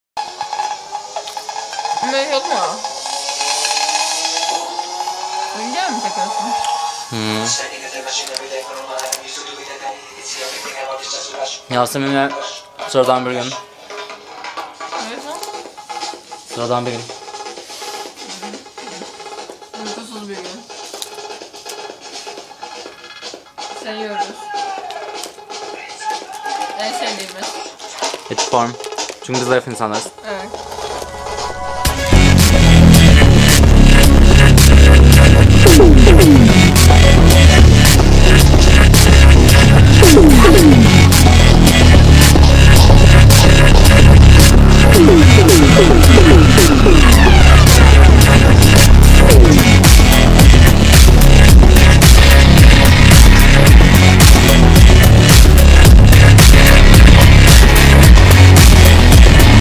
Zayıf İnsanlarız
An ordinary day in my friends apartment.